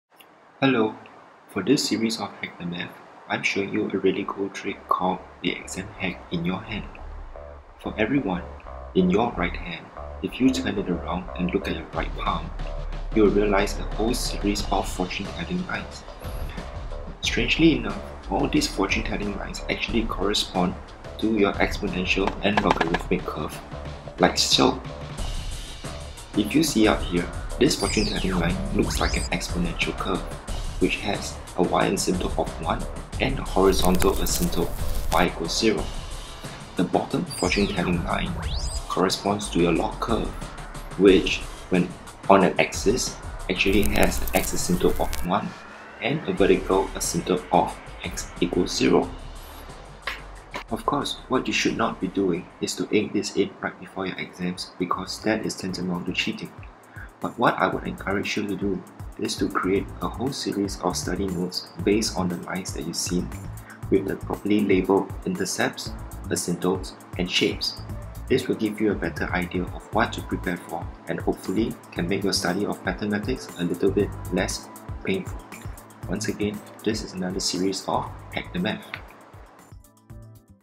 HackTheMath: The exam hack in your Right Palm
How fortune-telling lines on your right palm can help you quickly recall the default logarithmic and exponential curves.